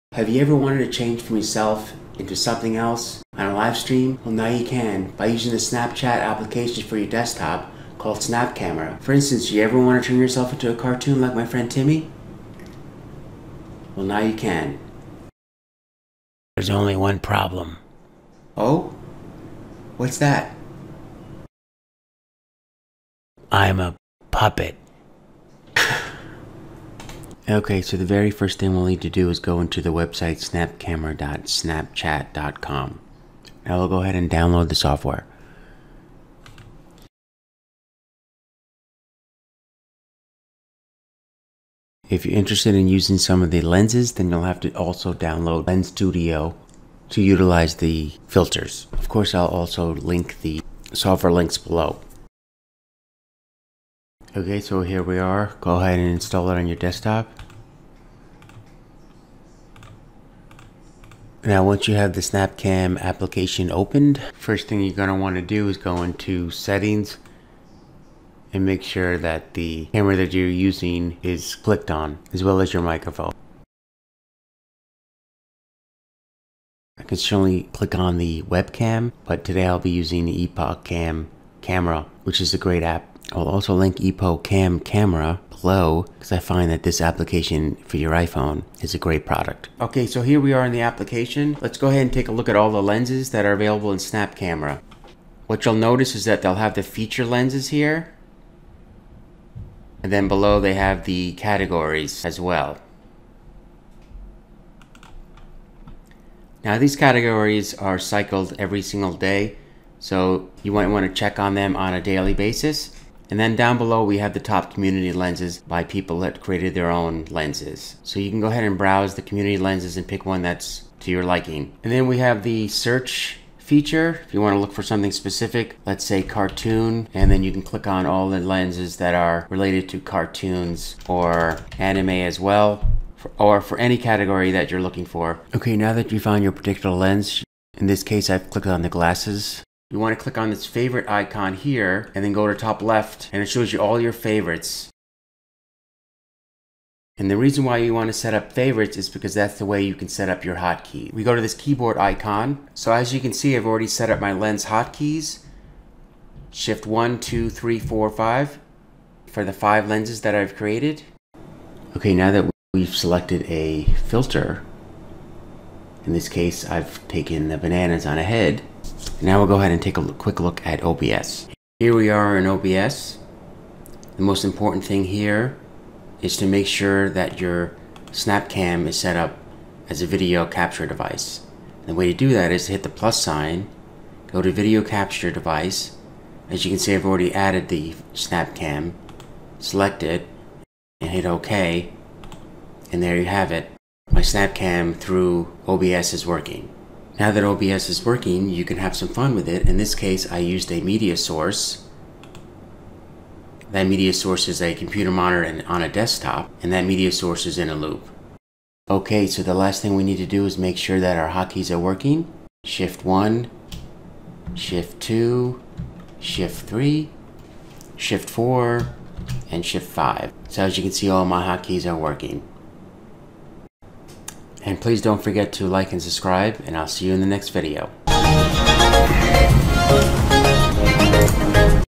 Change Your Face With Animated Characters On Your Live Streams For Free!
In this video,  you will see how to replace your face with all sorts of cool masks for your live streams, using SnapChat's Snap Camera app for Windows and Mac PCs.
You can use this method in OBS, Streamlabs OBS, Streamyard, Restream, Melon  ECamm, Zoom, Google Meet, Microsoft Teams & Discord.